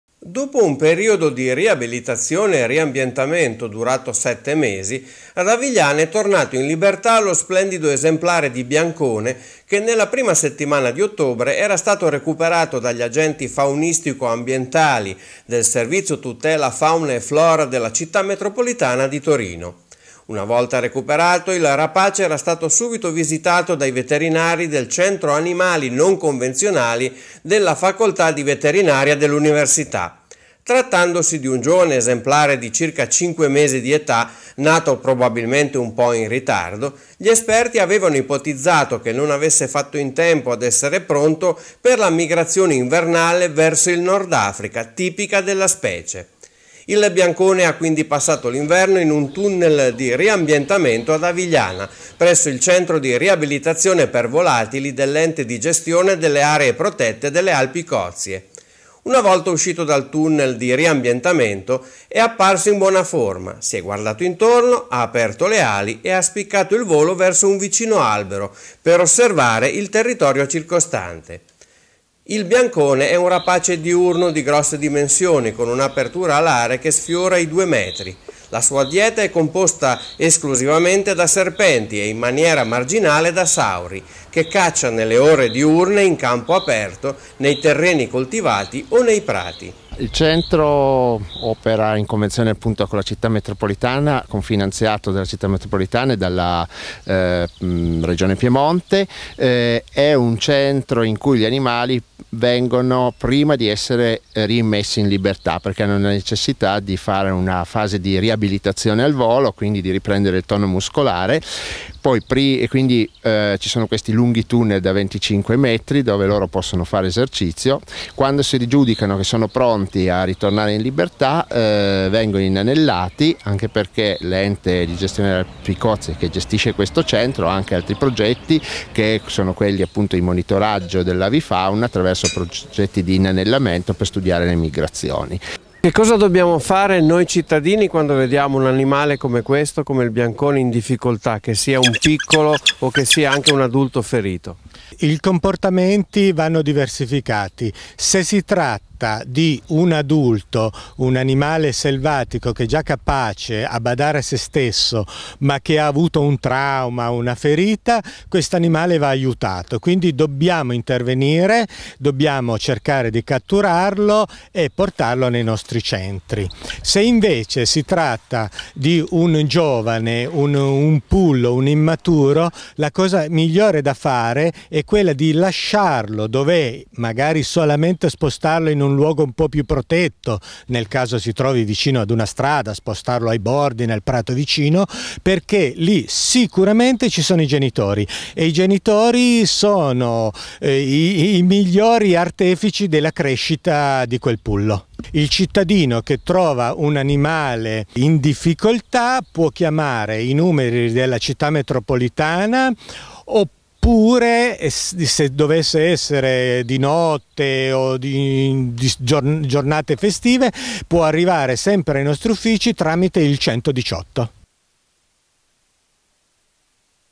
Libertà per il biancone
Ad Avigliana è tornato in libertà il giovane biancone nato in ritardoe recuperato nell'ottobre del 2015 nelle valli di Lanzo